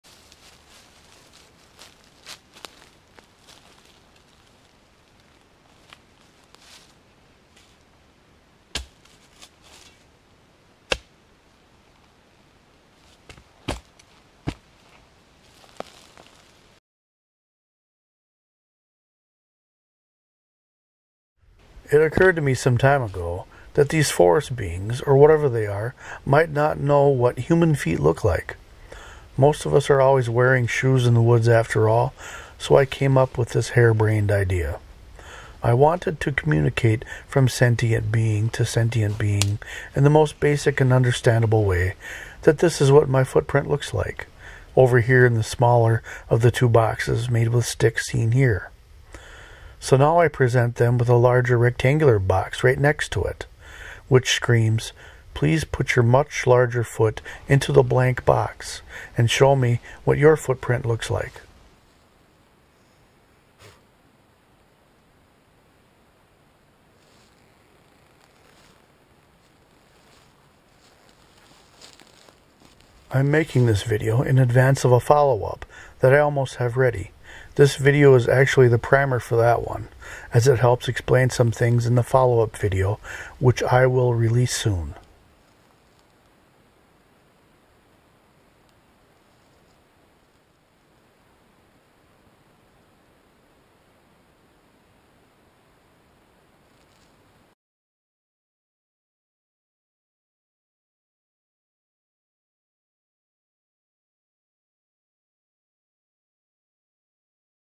THE REQUEST
Hair-brained idea of mine here, the audio that was left overnight at this spot did yield some 'Andre the Giant like' laughing though. video of that laughing